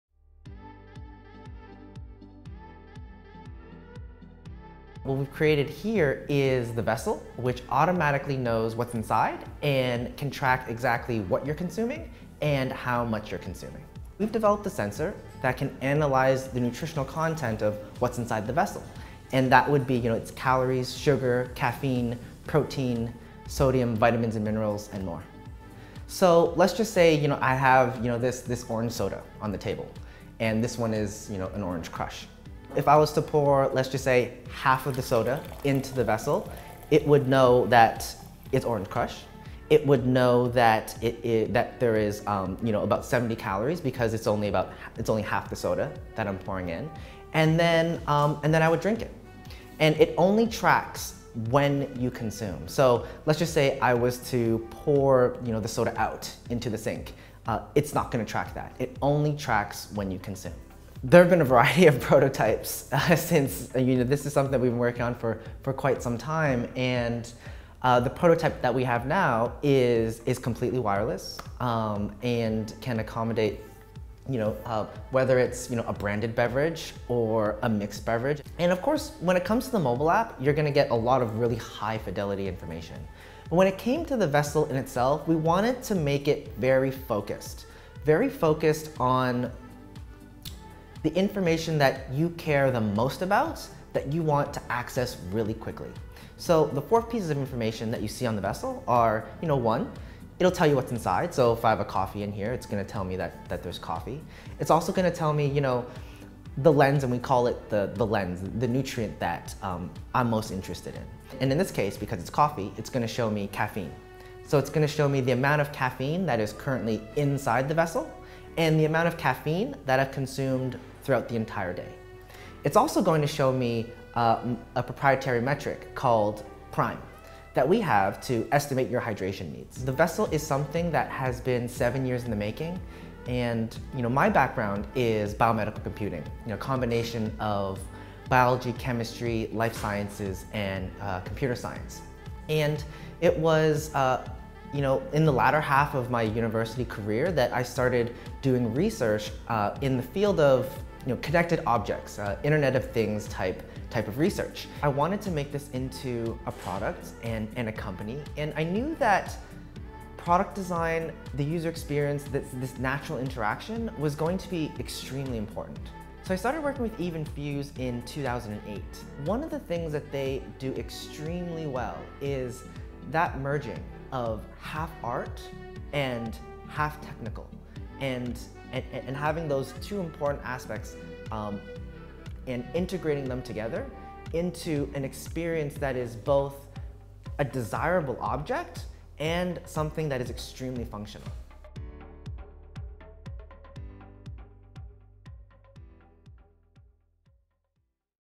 Vessyl: The Cup That Knows Its Contents | Forbes
Justin Lee and Jawbone designer Yves Behar teamed up to invent Vessyl, a smart cup equipped with sensors that can identify any beverage and track calories, caffeine, protein and hydration.